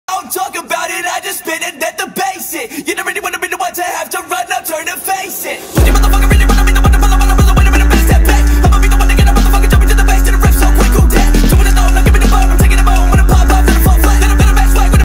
Small but terrible❤️ Grouse Locust
Grouse Locust, (family Tetrigidae), also called Pygmy Grasshopper
any of about 1,400 species of insects (order Orthoptera) that are small (about 15 mm [0.6 inch] long) the pygmy grasshopper has the forewings either reduced to small pads or absent. In addition, when not in flight, its folded membranous hindwings are protected by a pointed elongation of the thoracic shield @gistzillion